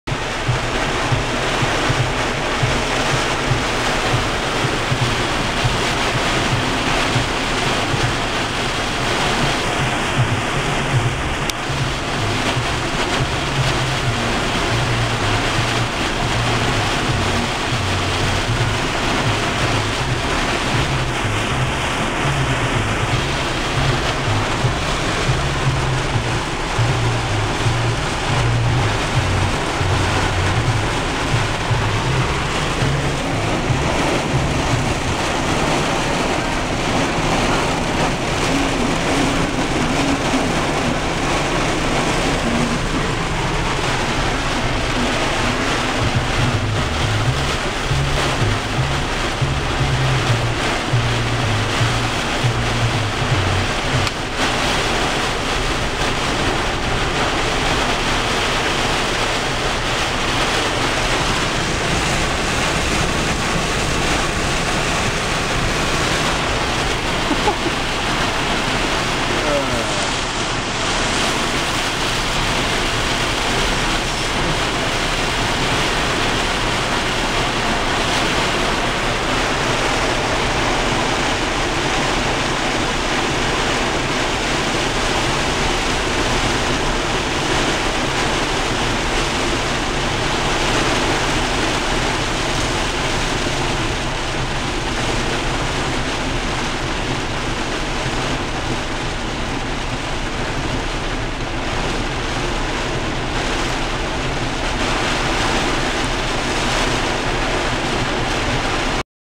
70 MPH Straight Line Winds Illinois Severe Storm Event
Sustained 70mph wind gusts in this "land hurricane" that rolled across central Illinois on June 7, 2015.